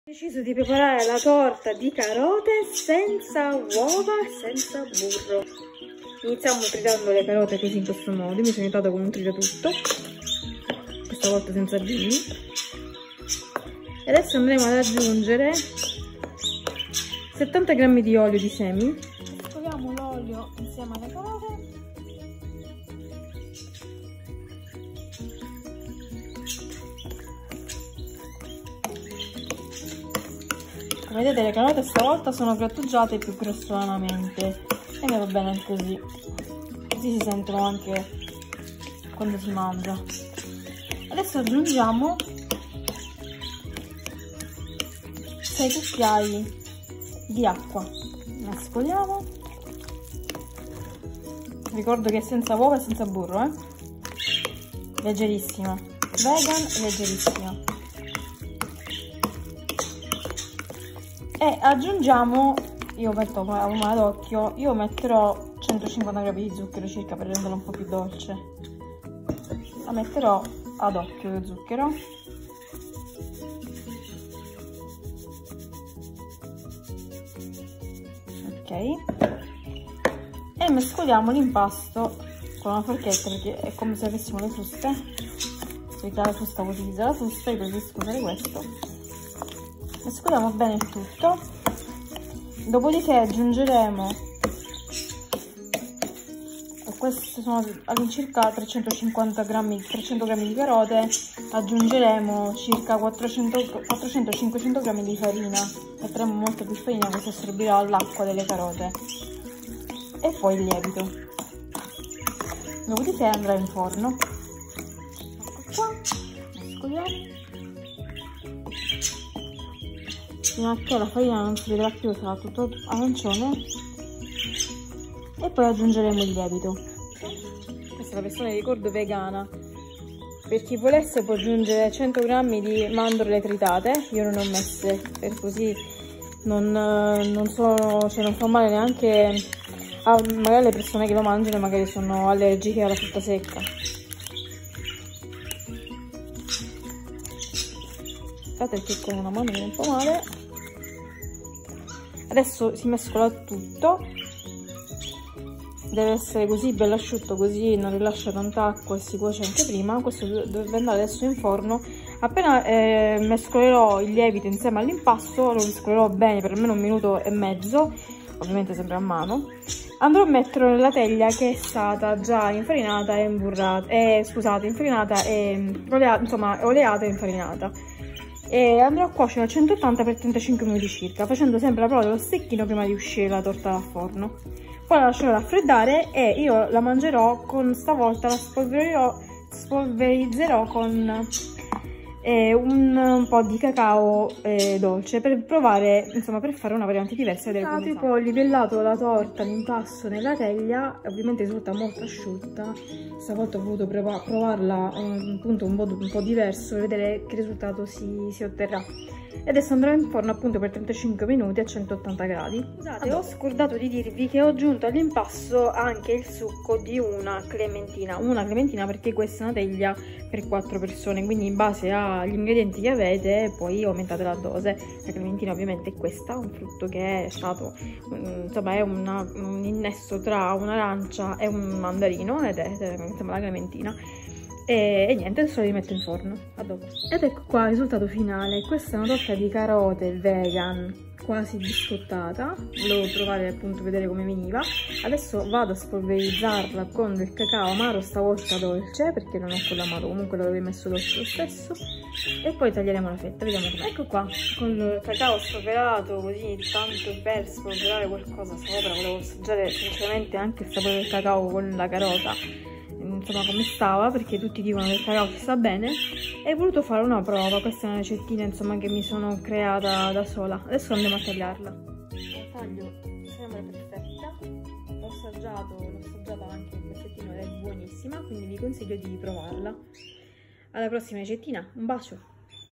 Torta carote VEGAN. Fatto in casa...PER ME!
Ingredienti:
500 gr di farina 00
300 gr circa di carote grattugiate
150 gr di zucchero
1 bustina di lievito
Succo di una clementina (o arancia)
Cacao in polvere dolce o amaro qb.
70 gr di olio di semi
6 cucchiai di acqua